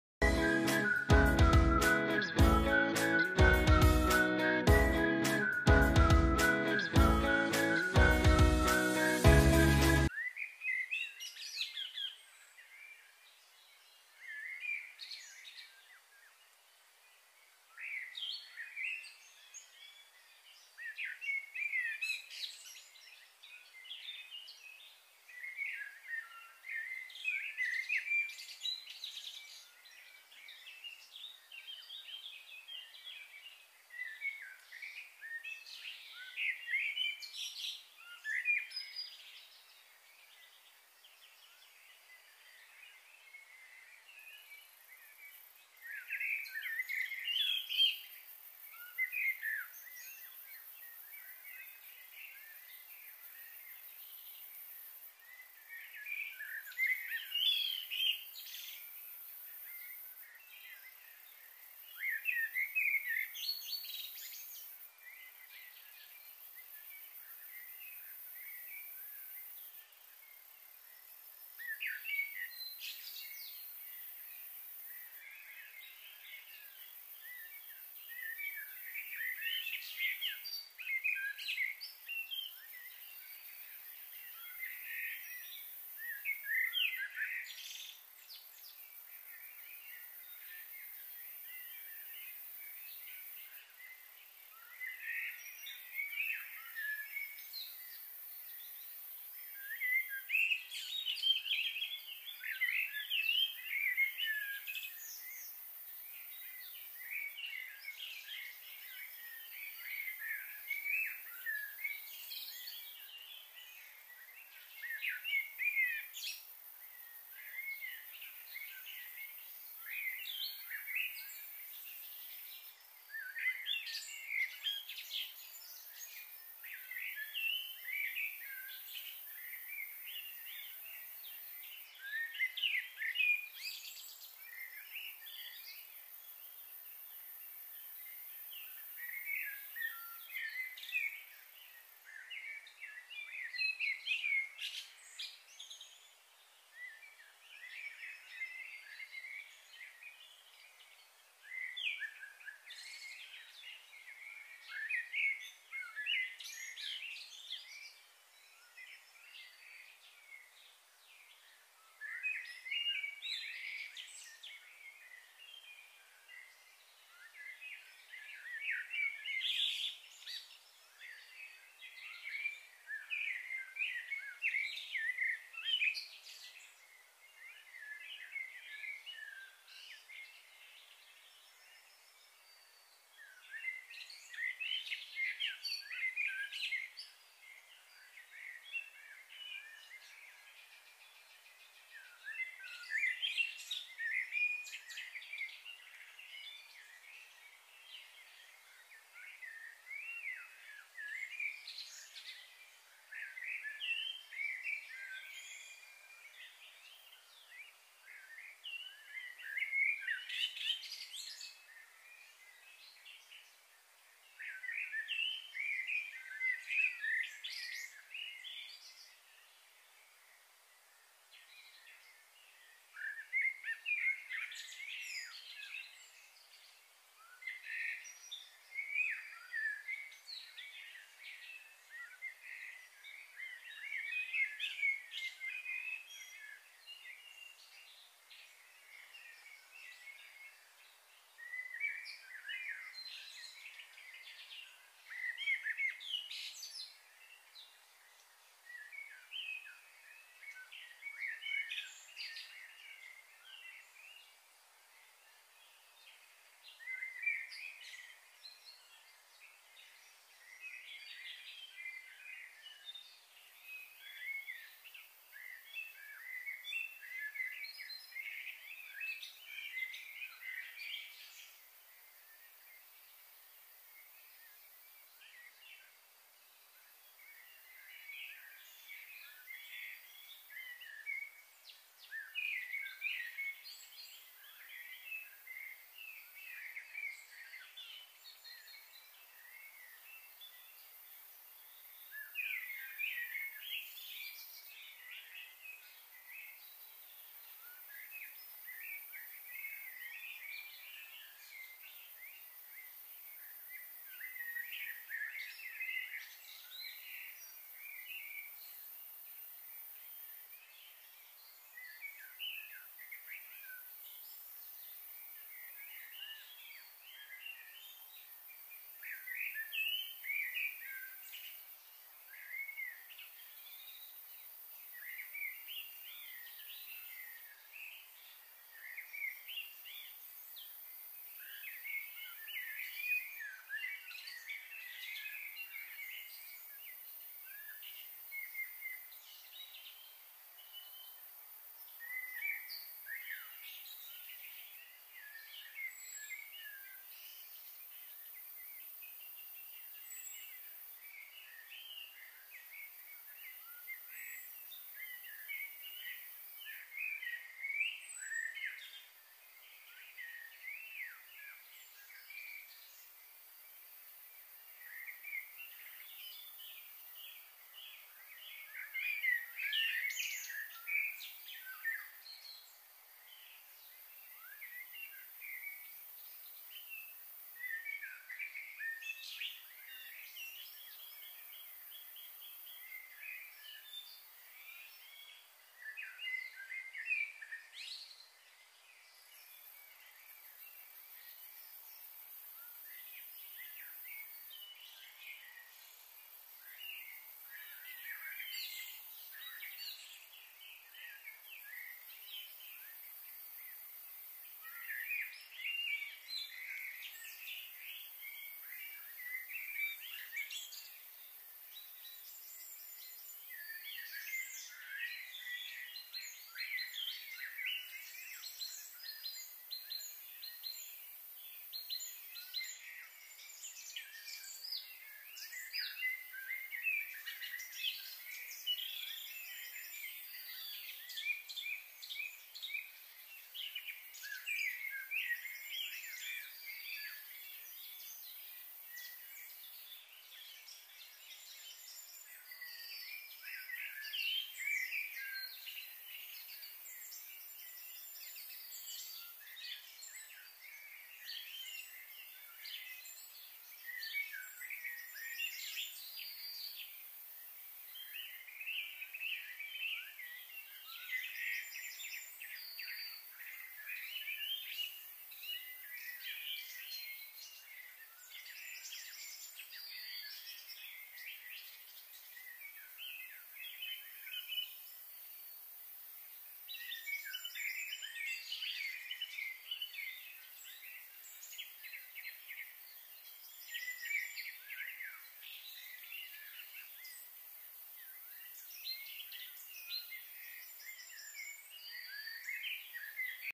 ASMR|D'jeeco Family|Gorilla|Taipei zoo|Jabali 超愛撒嬌，D'jeeco 在牆邊抱Jabali, Jabali 好幸福
20220317#15

Jabali loves to act like a baby, D'jeeco hugs Jabali by the wall, Jabali is so happy.|D'jeeco Family|Gorilla|Taipei zoo

🦍🚺Iriki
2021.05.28 Iriki gave birth of Ringo